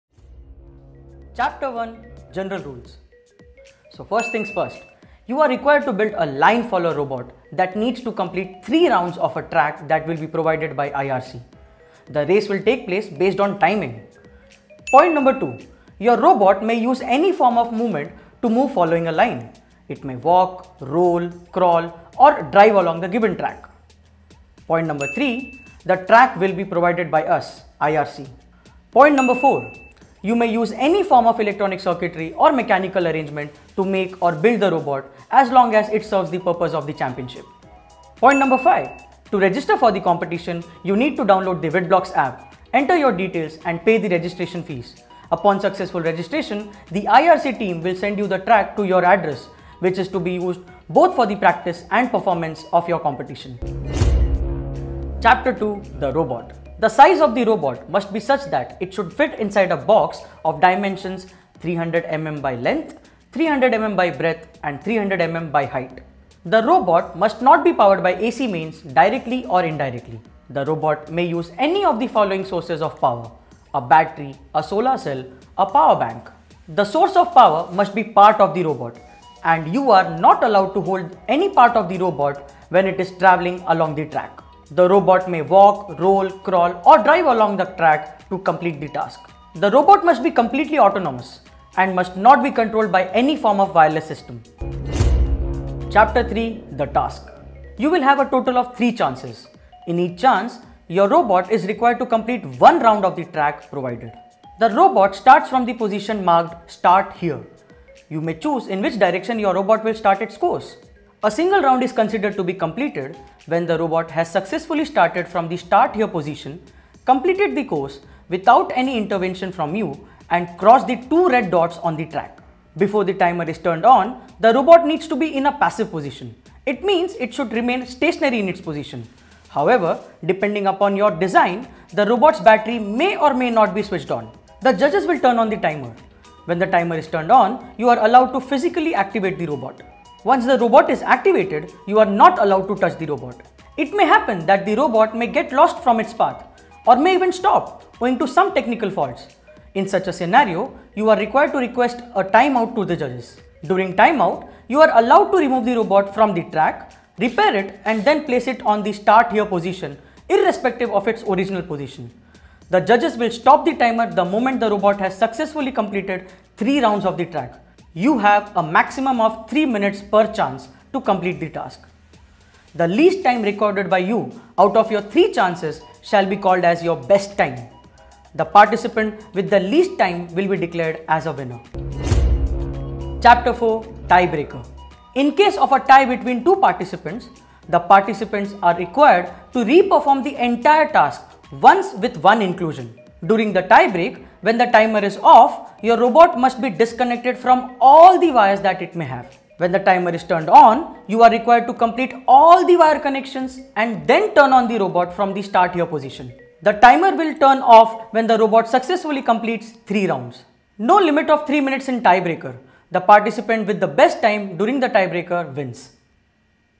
Rules of Indian Robotics Championship
See the rules you need to take care of while participating in IRC

Register yourself now on the WitBlox App and show your talent to the world